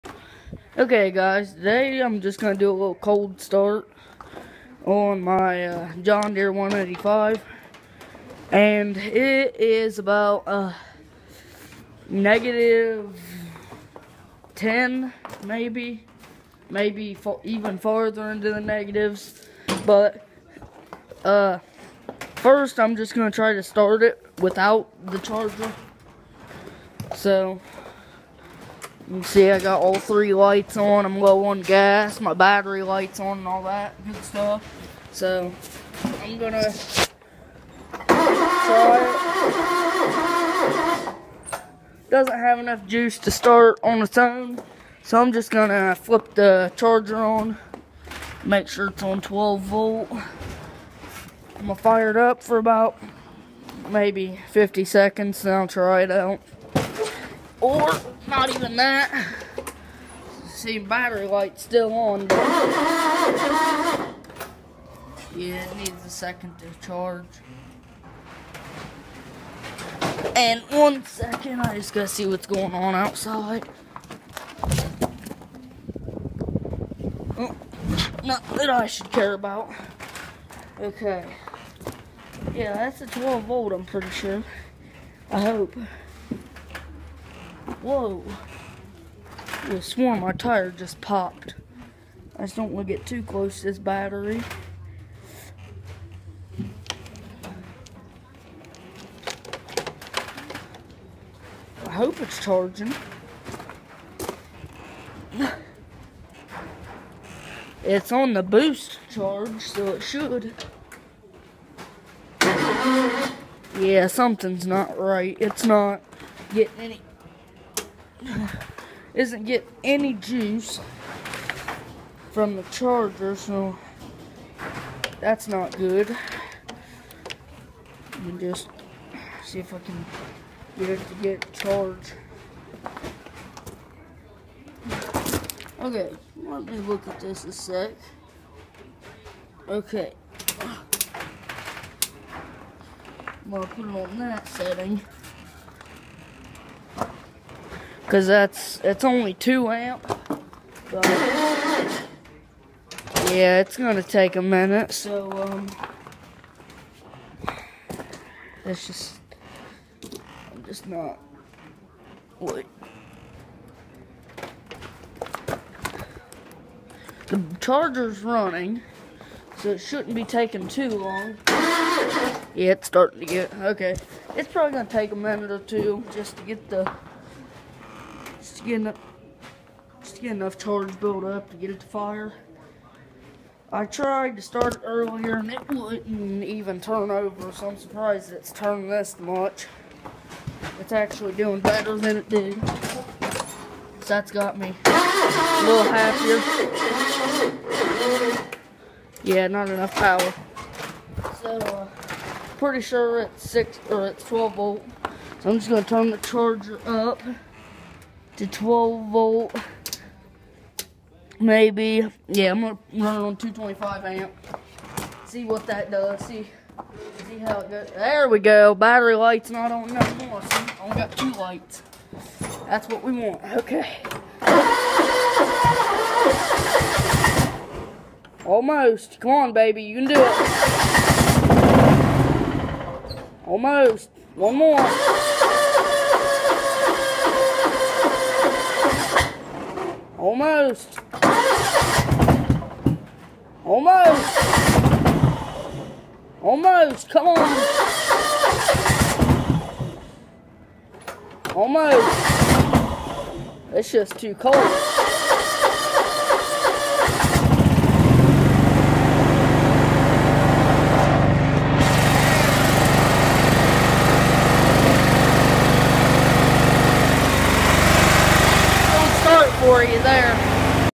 Cold start on my Jd 185
-10 and it took awhile to start but it started